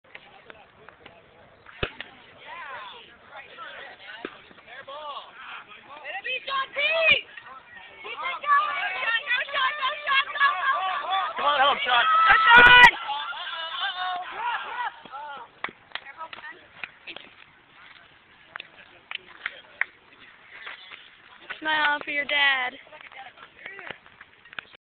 Home Run
I hit a homer and didn't realize it was being video recorded on a cell phone.  You can tell this because at the end I stand there smiling, waiting for the photo to be taken.  We won the game, btw.